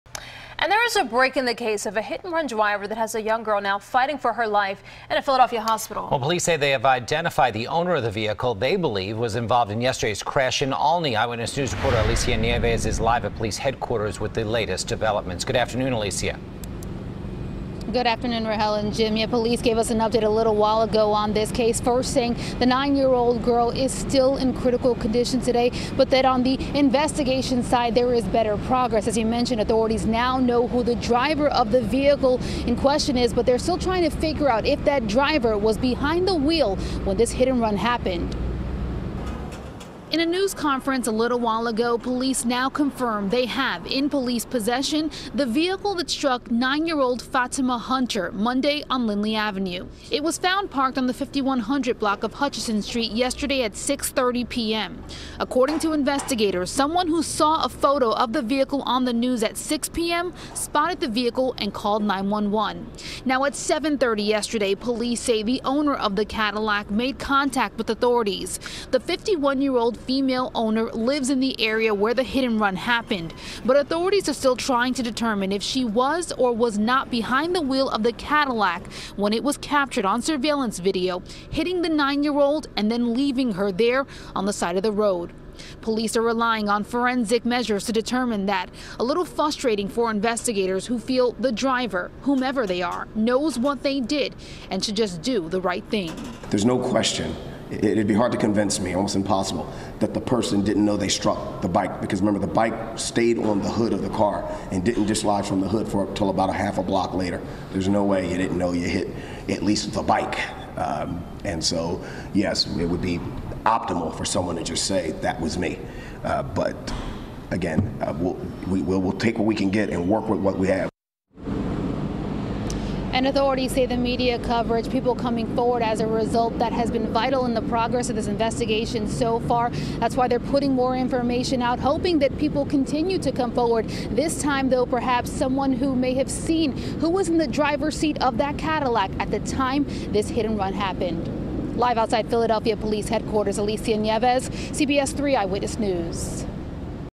Philadelphia Police Identify Owner Of Vehicle Involved In Hit-And-Run That Injured 9-Year-Old Girl
Alicia Nieves reports on the latest developments.